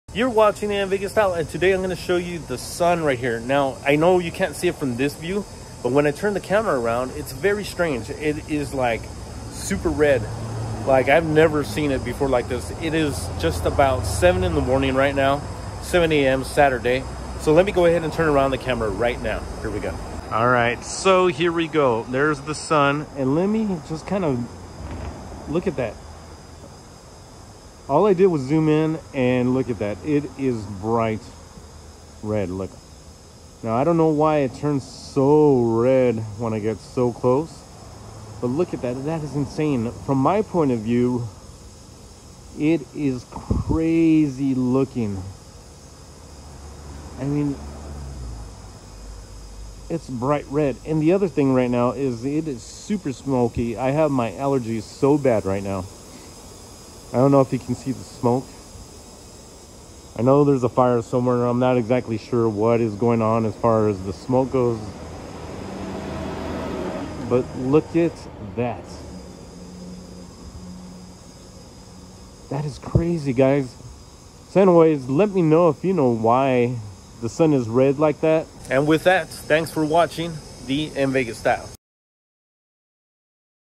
The Moon turns Red August 2021 in Las Vegas
I literally just woke up to a Red colored moon.  People have various theories about a red colored moon, what are your thoughts?

Is it because the moon covered the sun or is there another explanation for this?

Check out my gaming videos here or and Earn Lbry Coin by Viewing or Creating Content on Odysee: